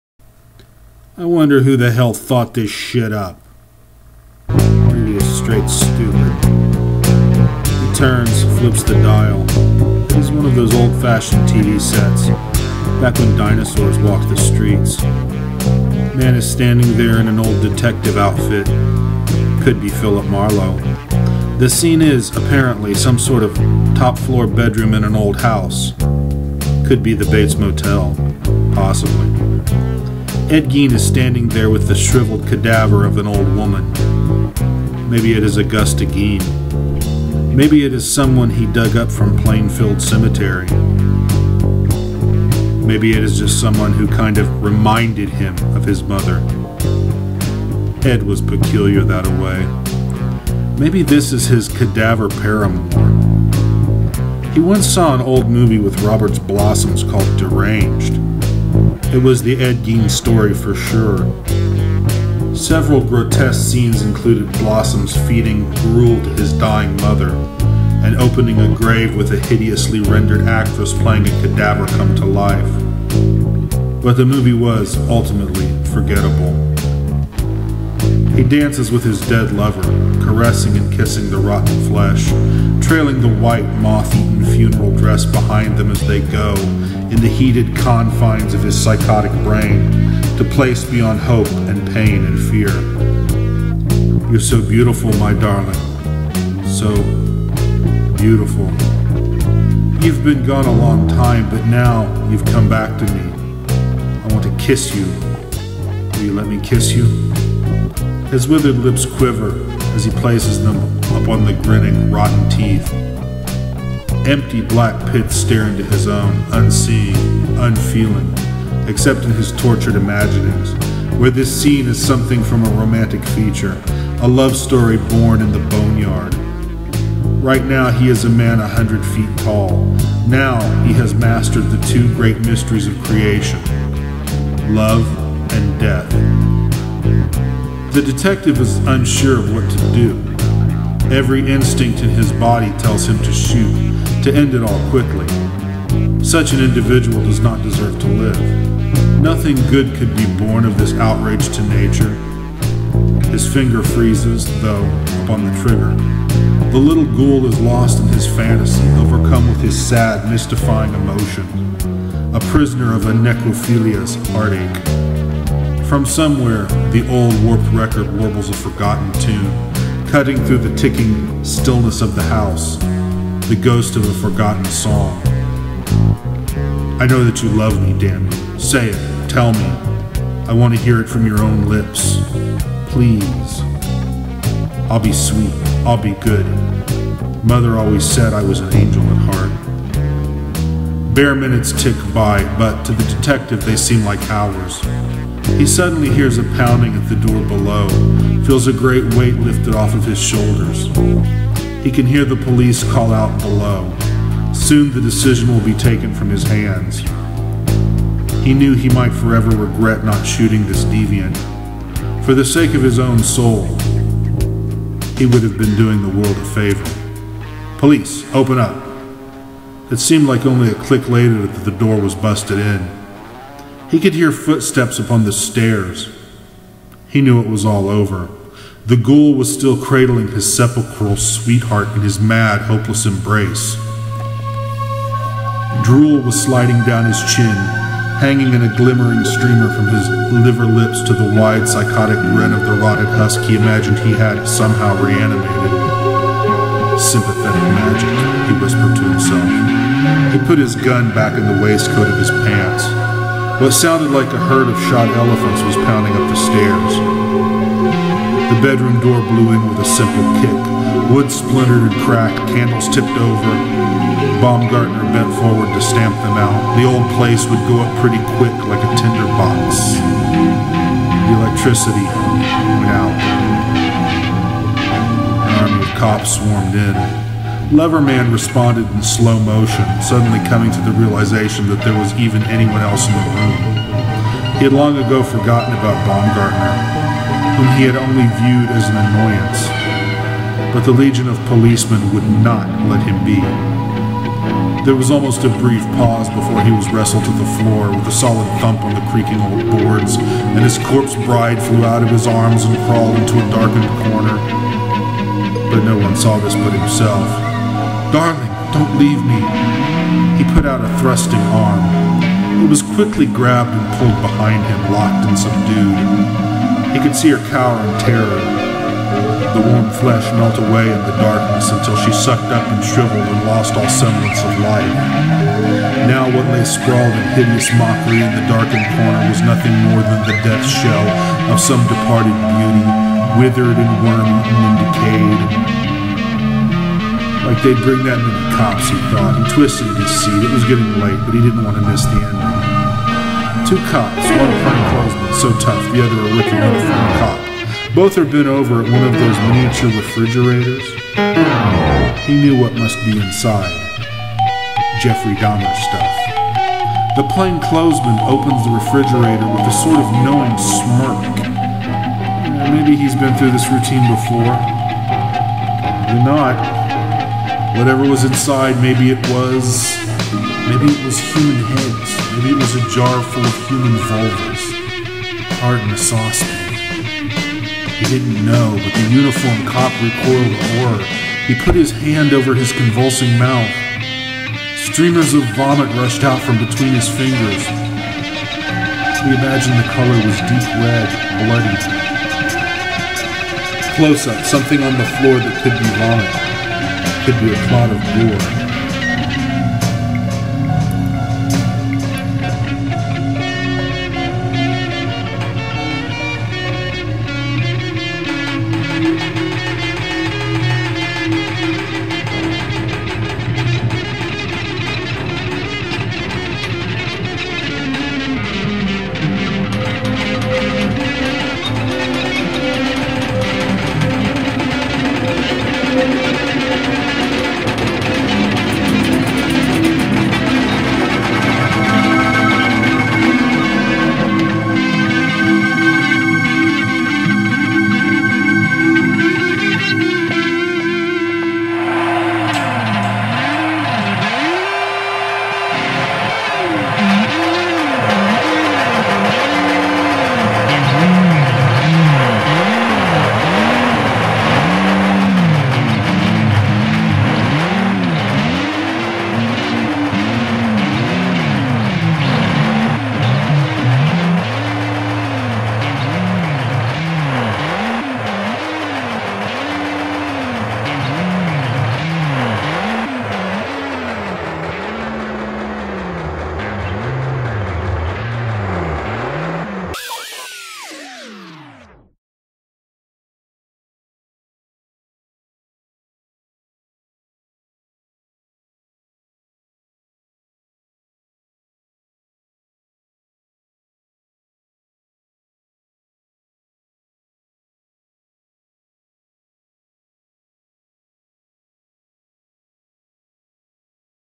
Tom Baker - An Ed Gein Movie (audiobook)
Text by Tom Baker:

–I wonder who the hell thought this shit up?

–Movie is straight stupid.

He turns, flips the dial. It is one of those old-fashioned TV sets, back when dinosaurs walked the streets.
Man is standing there in an old detective outfit. Could be Philip Marlowe. The scene is, apparently, some sort of top floor bedroom in an old house. Could be the Bates Motel. Possibly.

Ed Gein is standing there with the shriveled cadaver of an old woman. Maybe it is Augusta Gein. Maybe it is someone he dug up from Plainfield Cemetery.